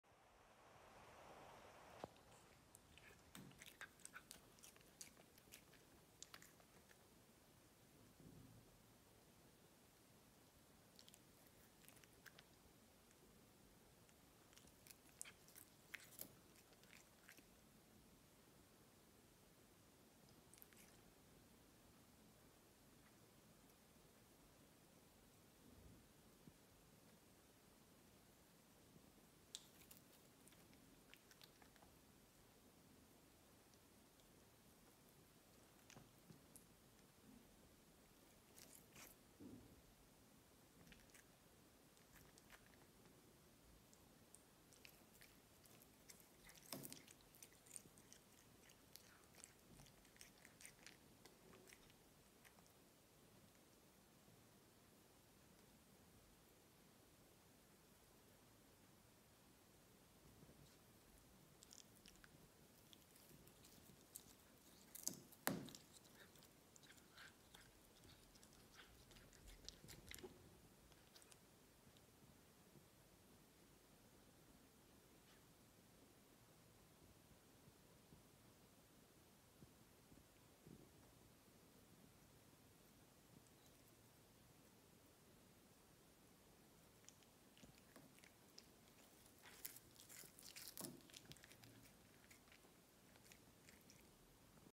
Katze verspeist Gras
Katze verspeist Gras - wie süß 🥰